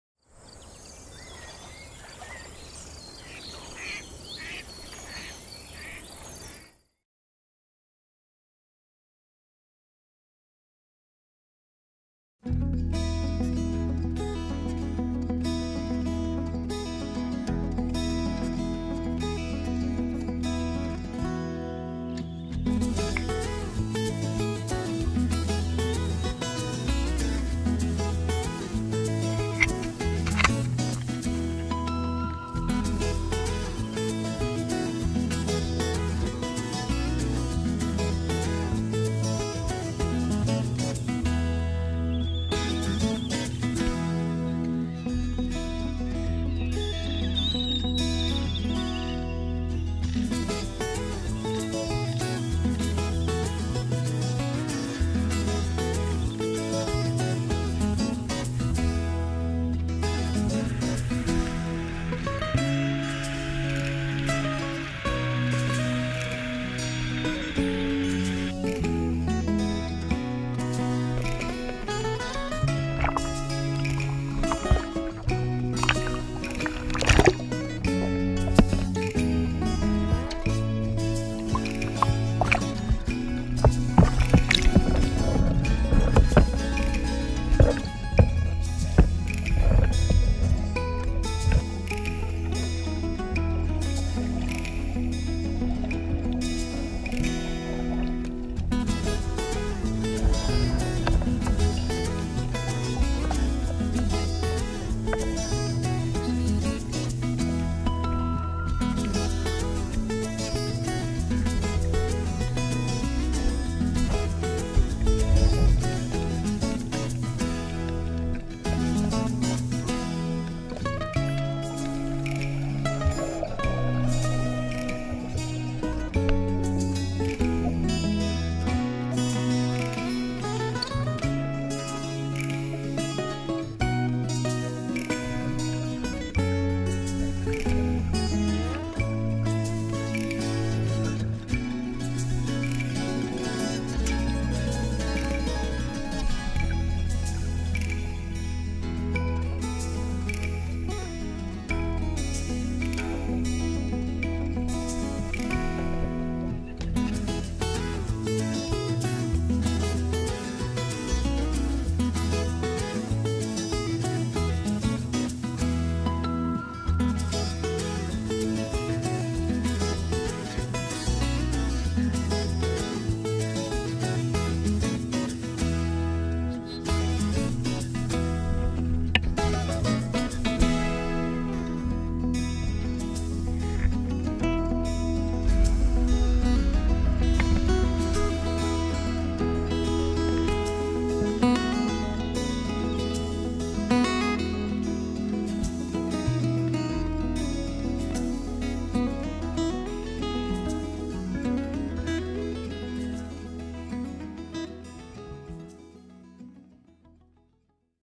Midway Day in the Life
Day in the life of USFWS employees working on aquatic resources surveys at Midway Atoll National Wildlife Refuge